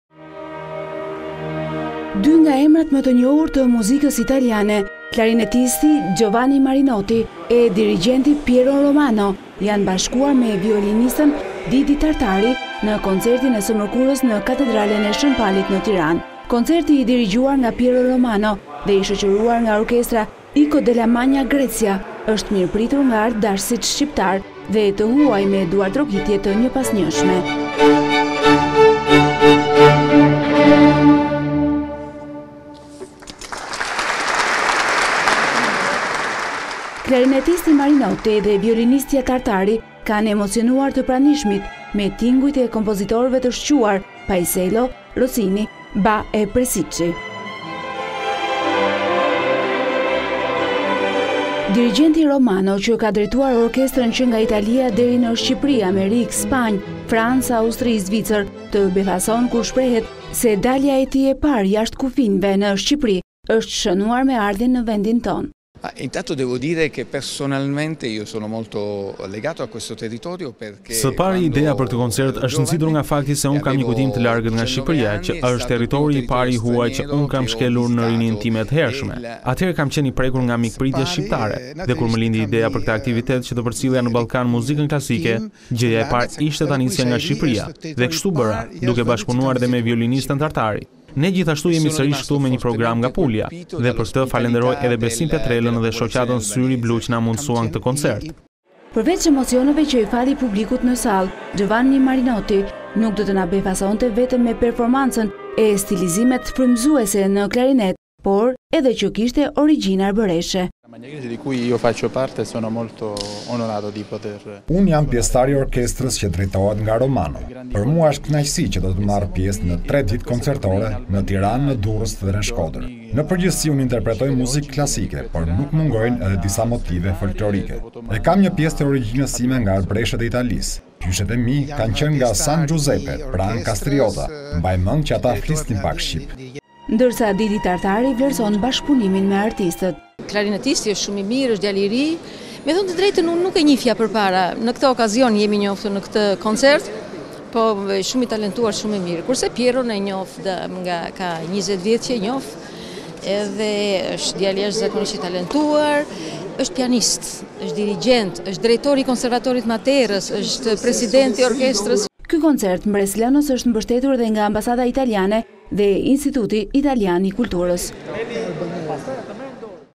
Report TV - Marinotti dhe Romano, virtuozët italianë nostalgji për Shqipërinë
Dy nga emrat më të njohur të muzikës italiane klarinetisti Giovanni Marinotti e dirigjenti Pierro Romano janë bashkuar me violinisten Didi Tartari në koncertin e së mërkurës në katedralen e Shën Palit në Tiranë.

Koncerti i dirigjuar nga Piero Romano dhe i shoqëruar nga orkestra ICO della magna Grecia është mirëpritur nga artdashësit shqiptarë dhe të huaj me duartrokitje të njëpasnjëshme.

Klarinetisti Marinotti dhe violinistja Tartari kanë emocionuar të pranishmit me tingujt e kompozitorëve të shquar Paisello, Rossini, Bach e Presicci.

Dirigjenti Romano që ka drejtuar orkestra që nga Italia deri në Shqipëri, Amerikë, Spanjë, Francë, Austri, Zvicër, të befason kur shprehet se dalja e tij e parë jashtë kufijve në Shqipëri është shënuar me ardhjen në vendin tonë.